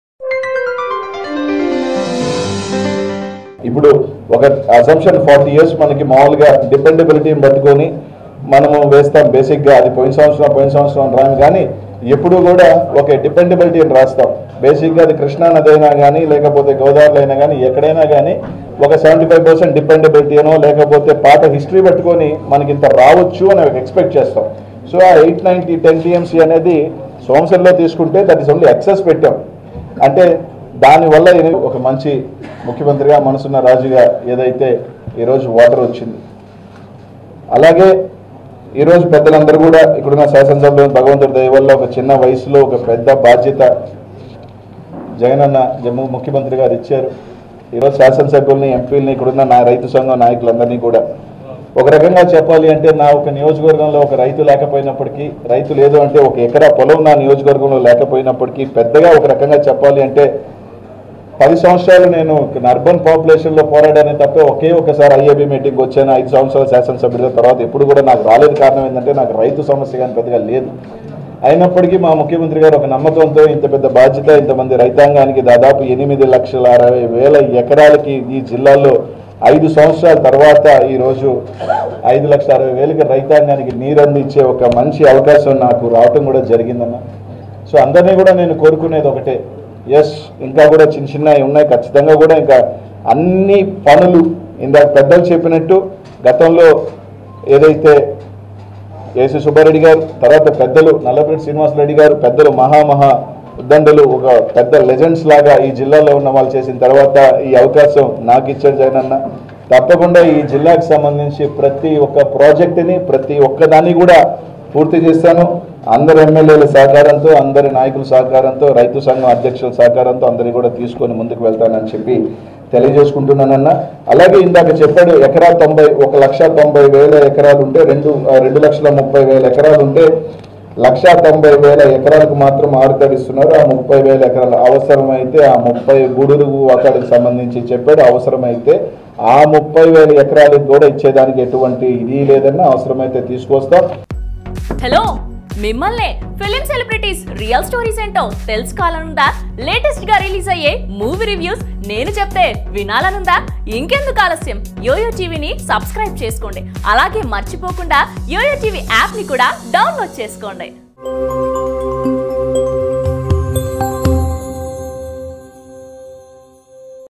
AP Minister Anil Kumar Yadav About Water Resources | Nellore News | AP CM YS Jagan | YOYO AP Times
AP Minister Anil Kumar Yadav About Water Resources, Nellore News, AP CM YS Jagan